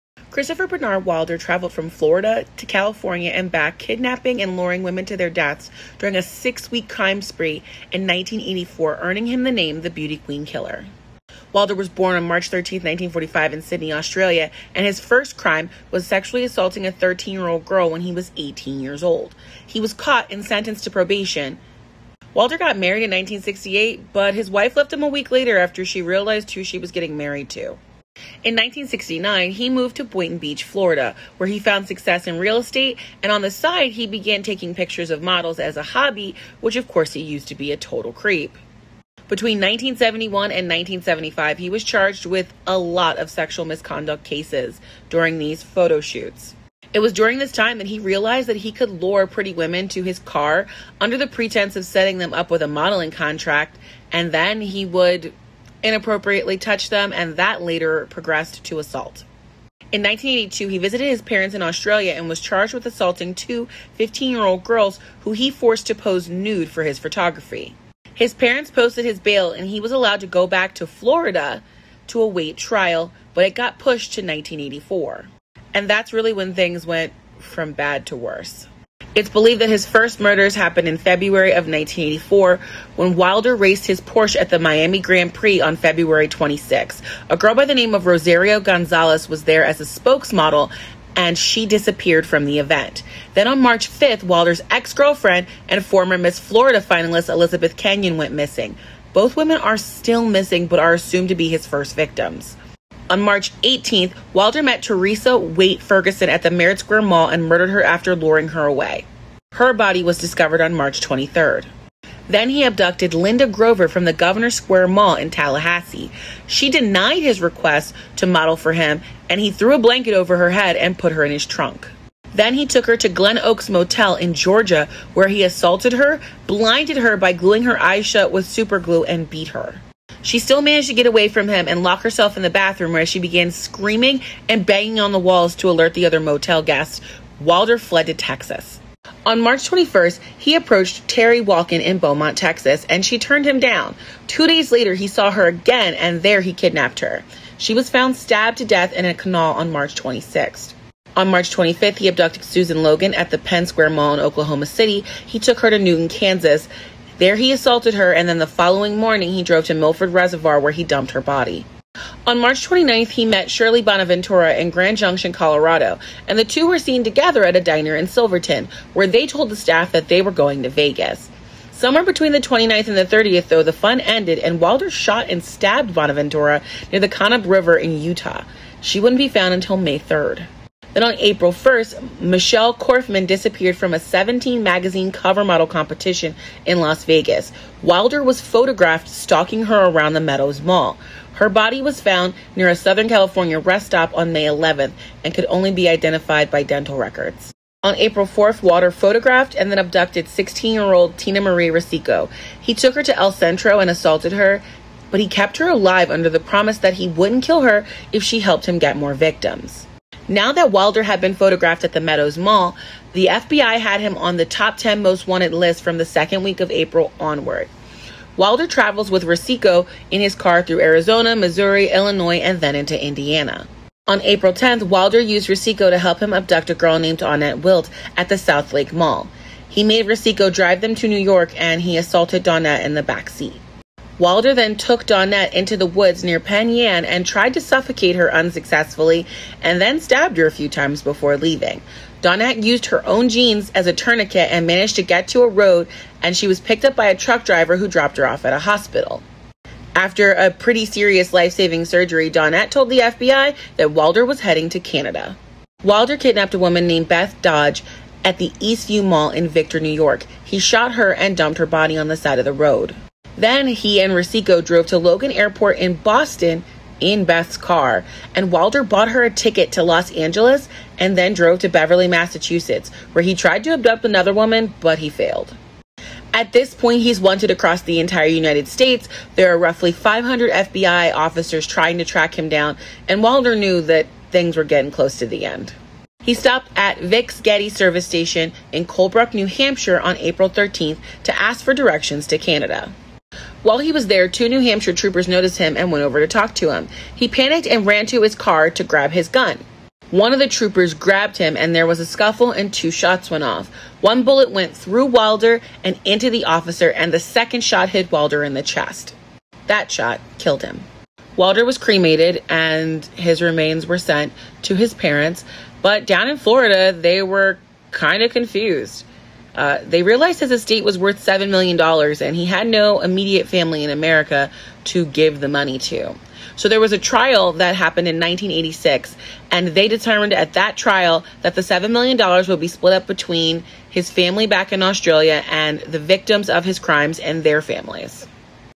Christoper Wilder's 6-week cross country murder spree earned him the name The Beauty Queen Killer
Since Tiktok banned me for 3 days over the Mickey Chervenak video(even though my appeal was granted) enjoy this YT exclusive video that's a little longer than the norm.

This is the tale of a man who was bad from the beginning. Christopher Bernard Wilder, an Australian man who ran from his crimes in Australia and then went on a horrific 6 week killing spree abducting young women across America.

Maybe I'll do more YT exclusive videos in the future. Sorry if the editting is awful. I'm not that great at it.